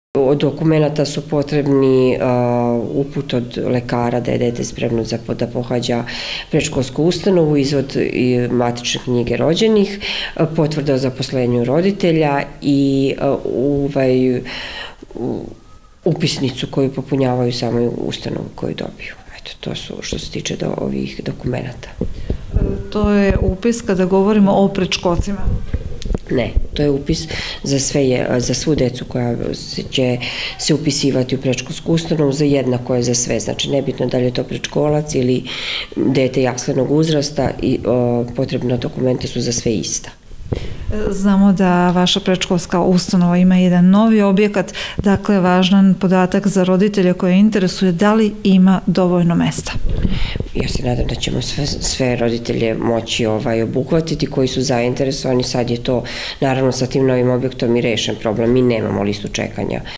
Upis u vrtić
Predškolska ustanova „Jelica Stanivuković Šilja” u Šidu uskoro počinje sa upisom dece u ovu ustanovu. Elektronski upis vršiće se od 15. aprila do 17. maja dok će konvencionalni biti obavljen od 7. do 31. maja. Upis se odnosi na sve starosne uzraste a, prema rečima direktorke ove ustanove Svjetlane Smolić, biće dovoljno mesta za svu decu. Saznajemo šta je potrebno od dokumenata